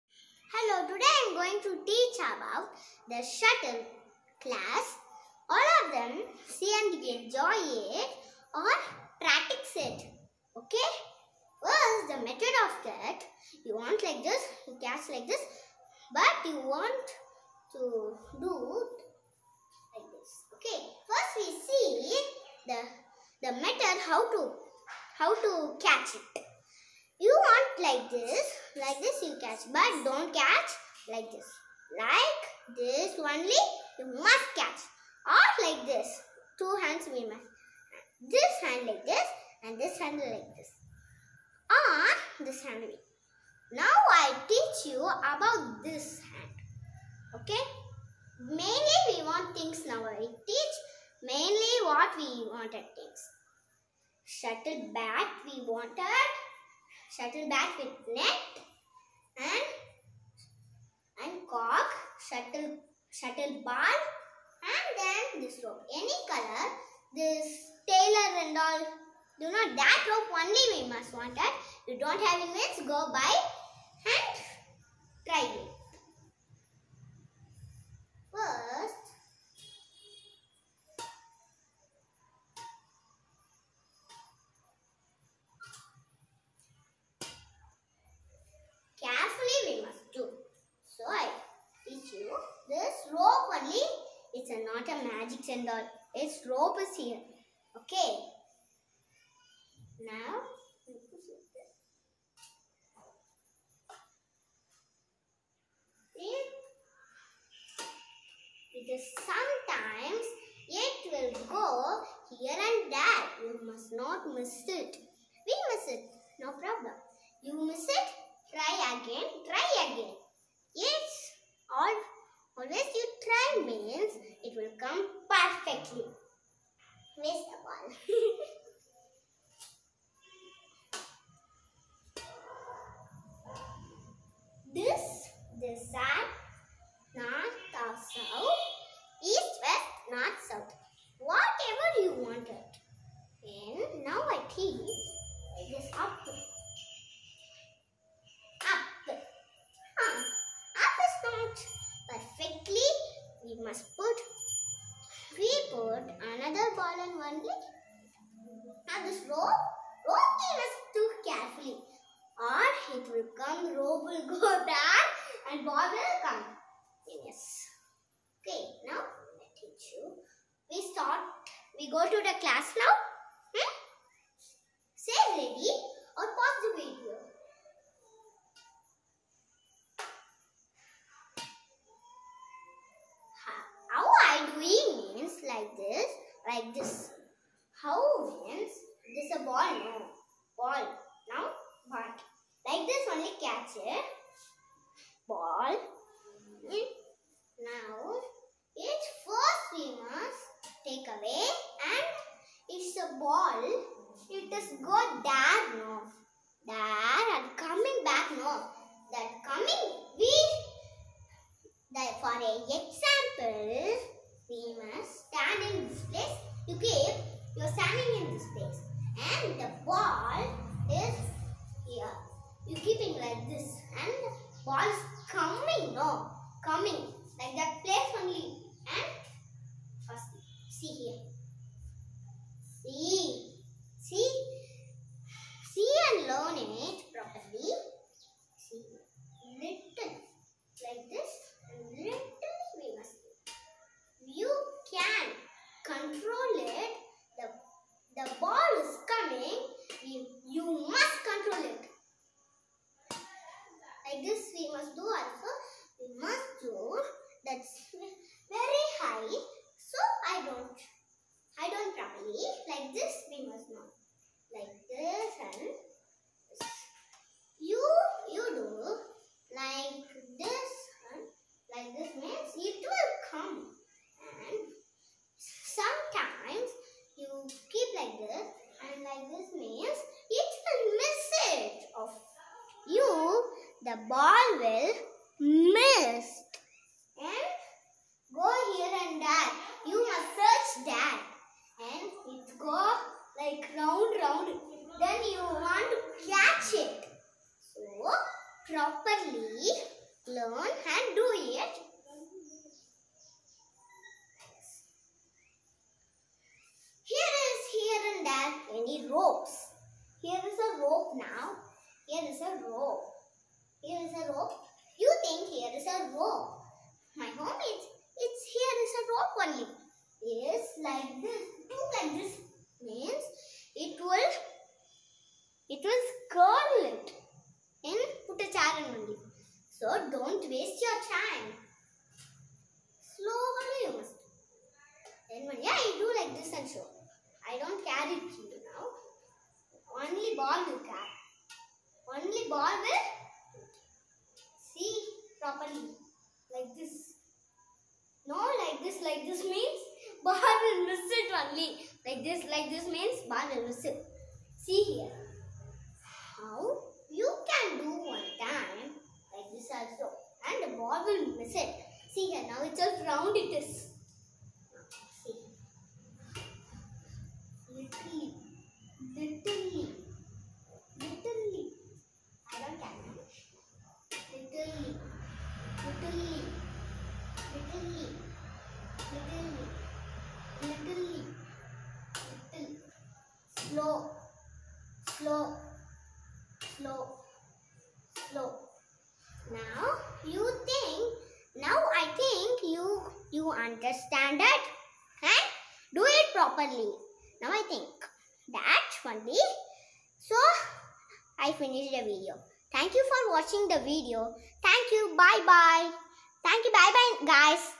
How to play shuttle basics
Maya is teaching badminton basics steps.